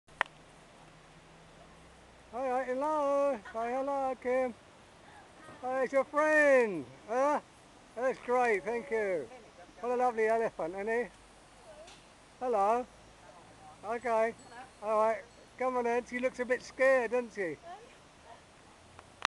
Sitting with elephant
sitting with an elephant at the masae elephant camp